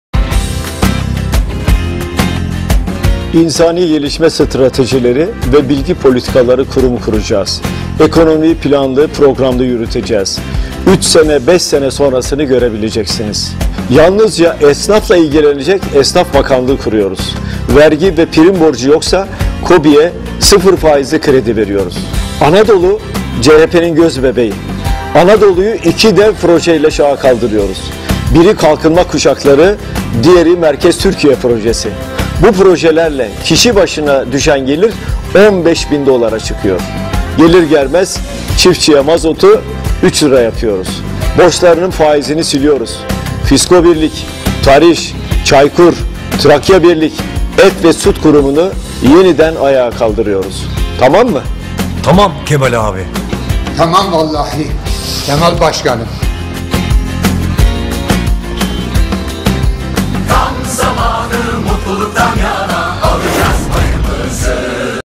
CHP'nin ekonomi vaatlerini biliyor musunuz?
CHP'nin ekonomi vaatlerini biliyor musunuz? CHP ekonomiyi nasıl düzeltecek merak ediyorsanız CHP'nin yeni reklam filmini izlemelisiniz.